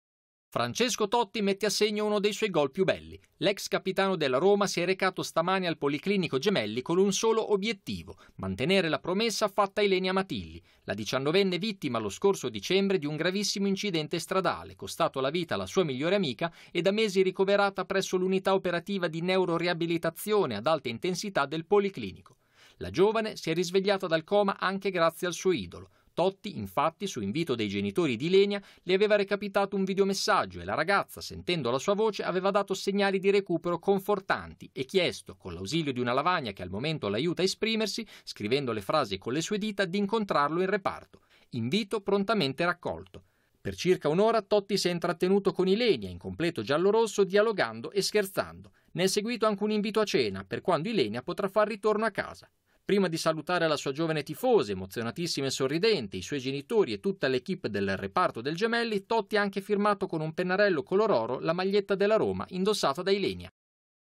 Totti visita la ragazza uscita dal coma grazie alla sua voce
Francesco Totti mette a segno uno dei suoi gol più belli. L'ex capitano della Roma si è recato stamane al Policlinico Gemelli con un solo obiettivo: mantenere la promessa fatta a Ilenia Matilli, la diciannovenne vittima lo scorso dicembre di un gravissimo incidente stradale costato la vita alla sua migliore amica e da mesi ricoverata presso l’Unità Operativa di Neuroriabilitazione ad Alta Intensità del Policlinico.
glb/mrv/red